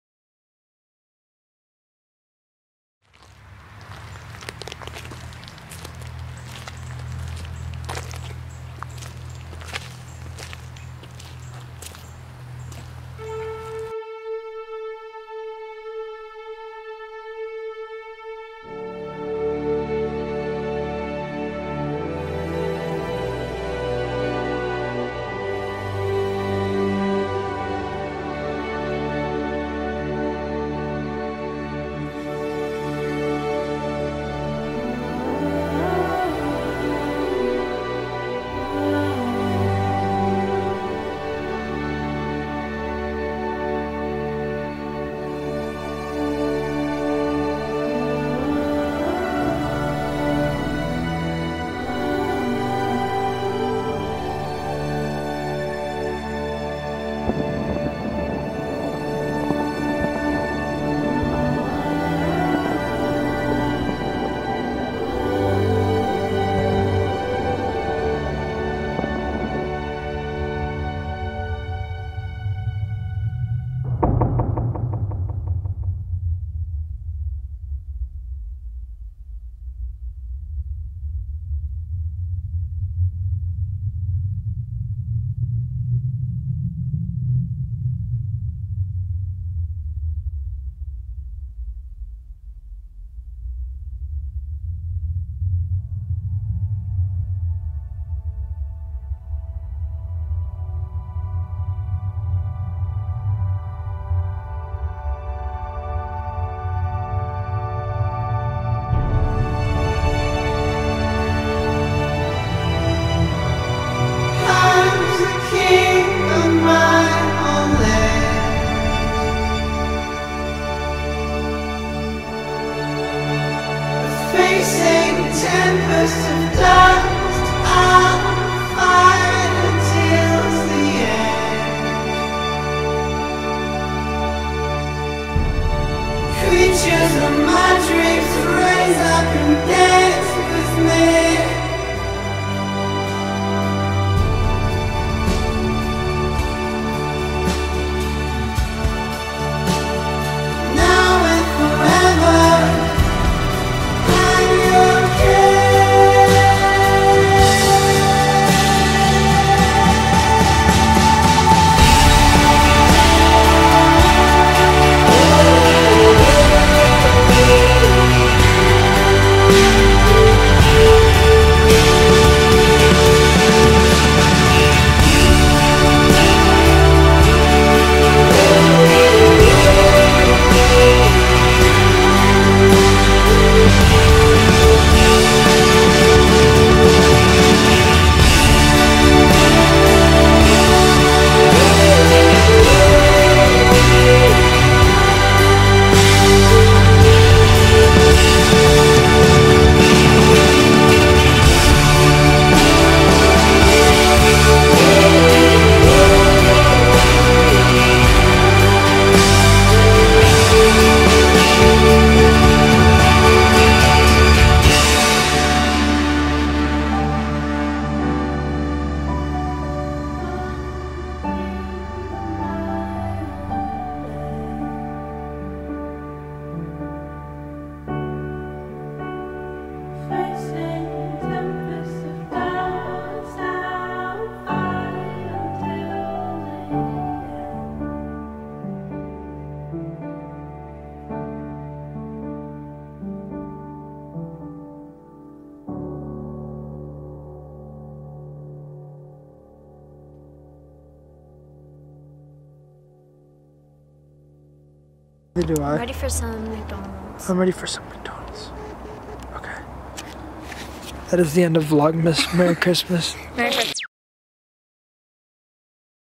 The Final Months of 2018 (A Short)
Here is the final recap of 2018. I wanted to make these recaps very fun, but also very honest at the same time. 2018 was a year full of ups and downs, and I feel like this final recap captured that perfectly, starting with the last few months then taking us back through everything captured over this last year. I hope you enjoy this final recap for 2018!:)